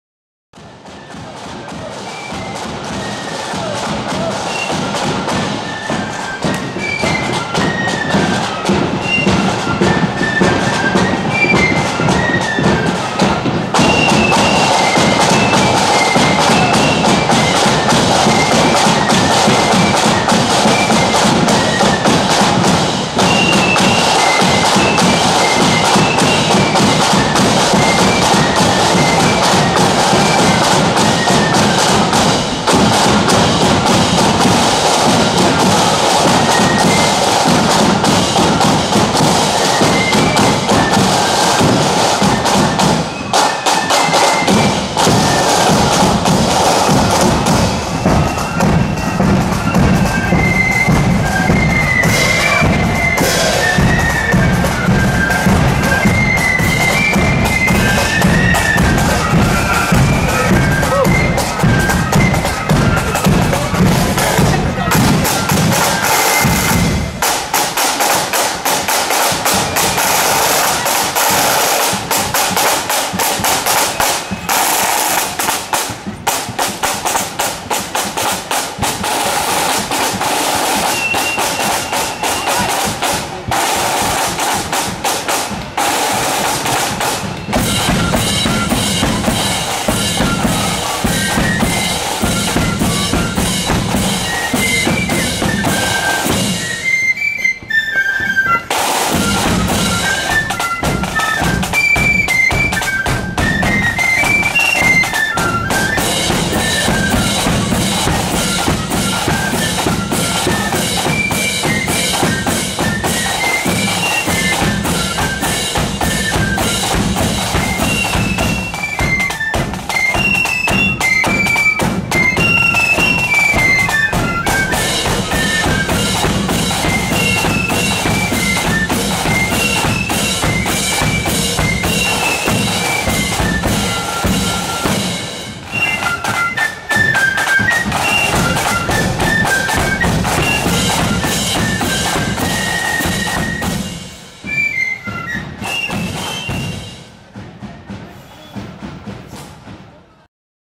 Flutes & Drum Donaghadee @ Bangor Protestant Boys Parade 2019
Flutes & Drum Donaghadee  on the march at Bangor Protestant Boys annual band parade Friday 6th September 2019